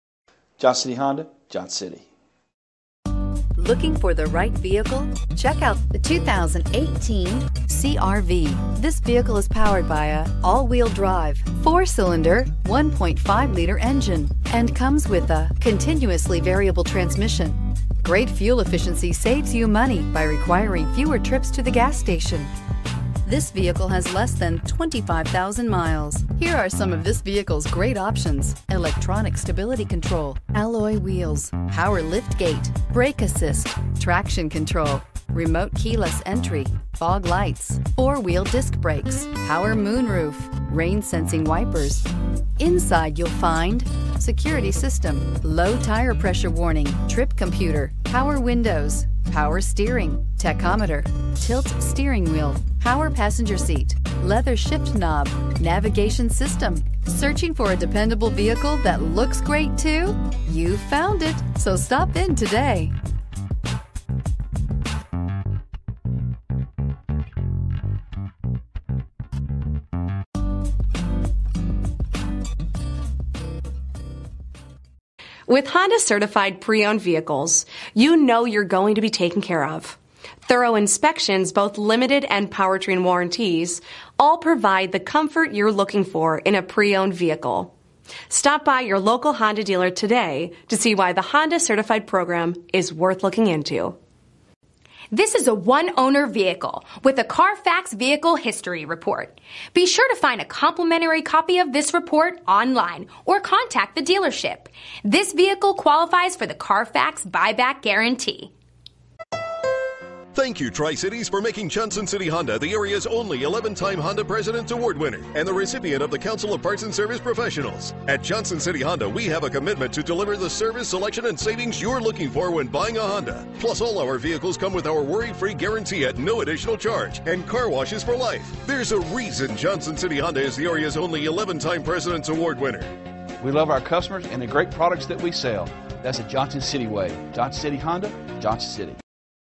2018 Honda CR-V Touring Used H48483A
CARFAX One-Owner. Clean CARFAX. Certified. White Diamond Pearl 2018 Honda CR-V Touring AWD CVT 1.5L I4 DOHC 16V CR-V Touring, 4D Sport Utility, 1.5L I4 DOHC 16V, CVT, AWD, White Diamond Pearl.Recent Arrival! 27/33 City/Highway MPGCertification Program Details: Roadside Assistance - Yes; Roadside Assistance for the duration of the Certified Pre-Owned Limited Warranty. Up to two complimentary oil changes within the first year of ownership. SiriusXM 90-day trial. Awards:  * Motor Trend Automobiles of the yearThe Johnson City Way.